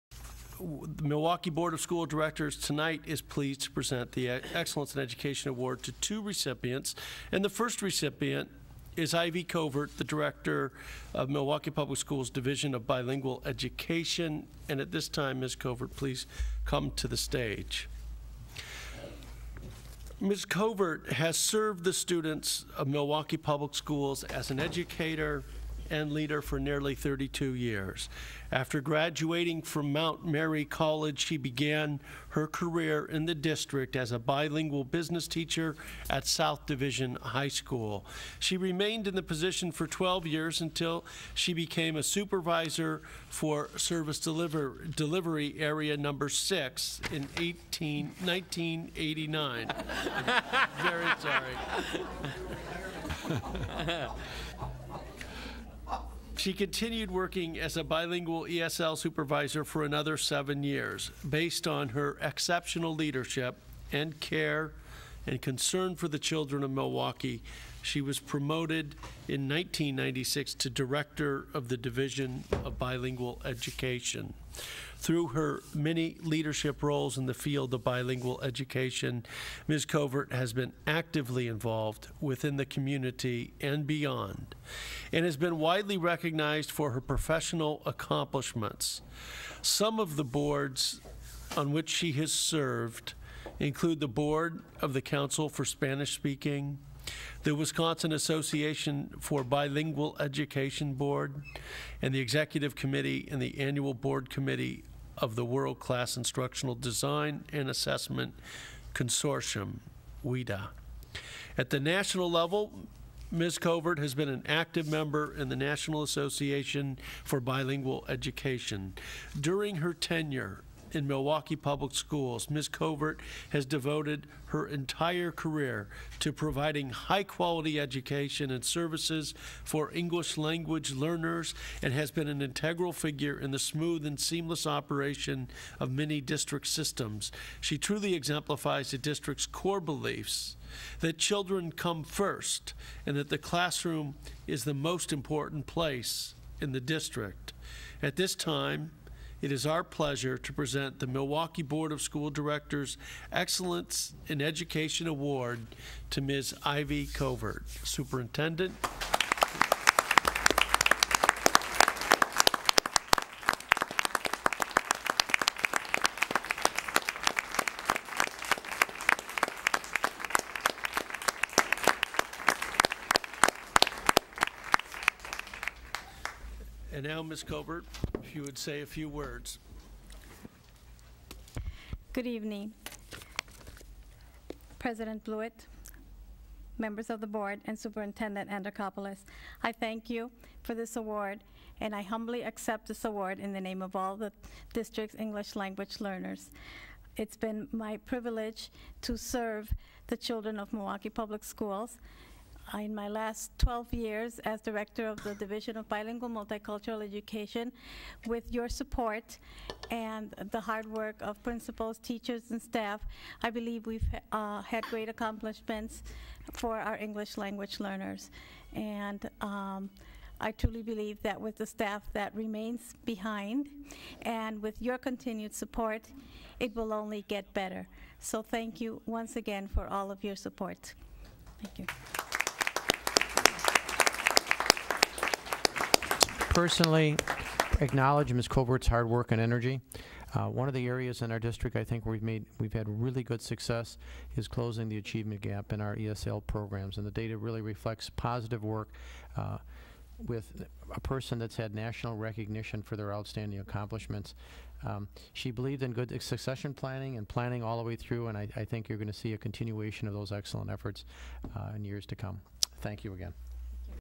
Ivy Covert: Excellence in Education
Ivy Covert has served the students of Milwaukee Public Schools as an educator and leader for nearly 32 years. After graduating from Mt. Mary College, she began her career in the district as a bilingual business teacher at South Division High School. She remained in this position for 12 years, until she became a supervisor for SDA (Service Delivery Area) #6 in 1989. She continued working as a bilingual/ESL supervisor for another seven years. Based on her exceptional leadership and care and concern for the children of Milwaukee, she was promoted in 1996 to Director of the Division of Bilingual Education.

Through her many leadership roles in the field of bilingual education, Ms. Covert has been actively involved within the community and beyond and has been widely recognized for her professional accomplishments. Some of the boards she has served on include the Board of the Council for Spanish Speaking; Board of the UW-Milwaukee Roberto Hernandez Center; the Wisconsin Association for Bilingual Education Board; Mt. Mary Educational Advisory Committee; and the Executive Committee and the Annual Board Committee of the WIDA (World-Class Instructional Design and Assessment) Consortium, appointed by the State Superintendent of Public Instruction. At the national level, Ms. Covert has been an active member in the National Association for Bilingual Education.

During her tenure in Milwaukee Public Schools, Ms. Covert has devoted her entire career to providing high-quality education and services for English-language learners and has been an integral figure in the smooth and seamless operation of many district systems. She truly exemplifies the districts core beliefs that children come first and that the classroom is the most important place in the district. The Milwaukee Board of School Directors recognize and honor Ivy Covert for her dedication, outstanding leadership, and commitment to excellence on behalf of the students of the Milwaukee Public Schools.